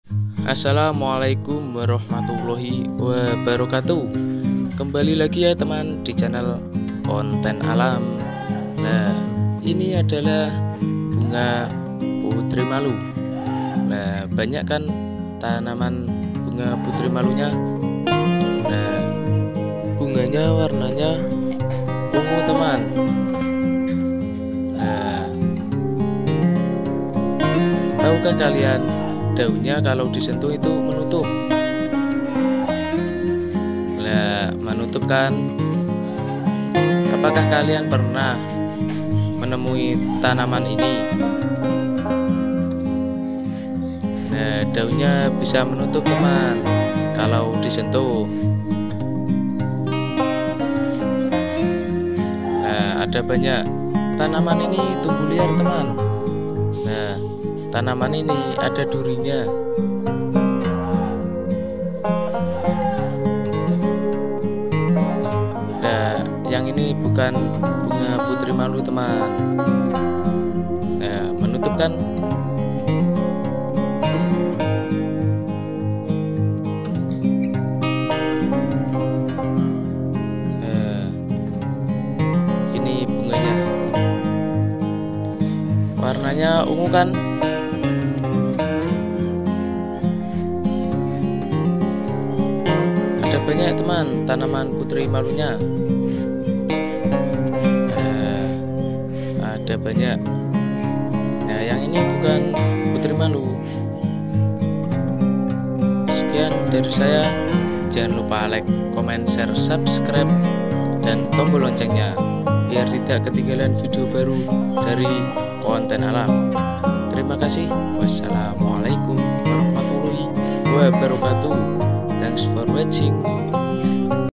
BUNGA INI DAUNNYA BISA MENUTUP KALAU DISENTUH😃
Terima kasih teman-teman karena kalian sudah menonton videoku